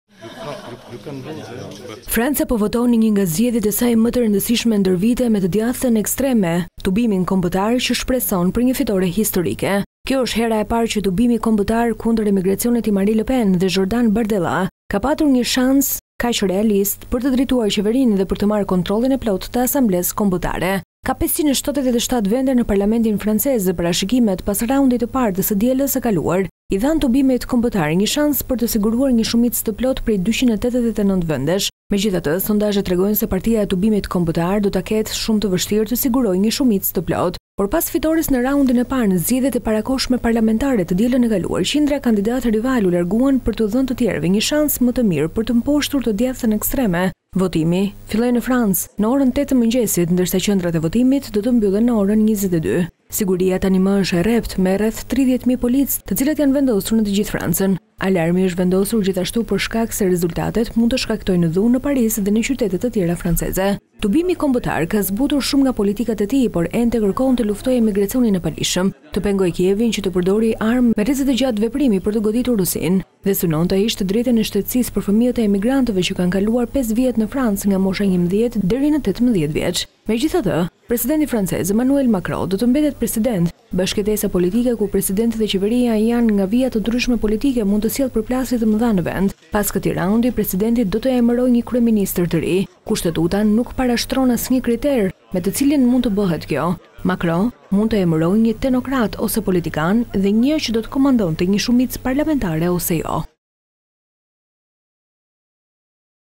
Klan News - E djathta ekstreme franceze kërkon fitoren
Franca po voton në një nga zgjedhjet e saj më të rëndësishme ndër vite, me të djathtën ekstreme që shpreson për një fitore historike.

Kjo është hera e parë që Tubimi Kombëtar kundër emigracionit i Marine Le Pen dhe Jordan Bardella ka pasur një shans realist për të drejtuar qeverinë dhe për të marrë kontrollin e plotë të Asamblesë Kombëtare.

Ka 577 vende në parlamentin francez dhe parashikimet pas raundit të parë të së dielës i dha Tubimit Kombëtar një shans për të siguruar një shumicë të plotë prej 289. Megjithatë, sondazhet tregojnë se Partia e Tubimit Kombëtar e ka të pamundur të sigurojë një shumicë të plotë.

Por pas fitores në raundin e parë në zgjedhjet e parakohshme parlamentare të dielën e kaluar, qindra kandidatë rivalë u larguan për t'u dhënë të tjerëve një shans më të mirë për të mposhtur të djathtën ekstreme. Votimi filloi në Francën kontinentale në orën 08:00 ndërsa qendrat e votimit do të mbyllen në orën 22:00.

Siguria ishte tashmë e rreptë dhe tani 30,000 policë janë vendosur për një periudhë tensioni të shtuar politik. Alarmi është vendosur në vend pasi rezultatet mund të shkaktojnë dhunë në Paris edhe qytete të tjera franceze.

Tubimi Kombëtar ka zbutur shumë nga politikat e tij, por ende kërkon të luftojë emigracionin e paligjshëm, do të pengojë Kievin që të përdor armë me rreze të gjatë të furnizuar nga Franca për të goditur Rusinë dhe synon të heqë të drejtën e shtetësisë për fëmijët e emigrantëve që kanë kaluar pesë vjet nga mosha 11 deri në 18 vjeç në Francë.

Megjithëse presidenti francez Emmanuel Macron do të mbetet president, bashkëjetesa politike ku presidenti dhe qeveria janë nga vija të ndryshme politike mund të sjellë përplasje të mëdha në vend.

Pas këtij raundi, presidenti do të emërojë një kryeministër të ri. Kushtetuta nuk parashtron asnjë kriter me të cilin mund të bëhet kjo. Macron mund të emërojë një teknokrat ose politikan, dhe një që do të komandonte një shumicë parlamentare, ose jo.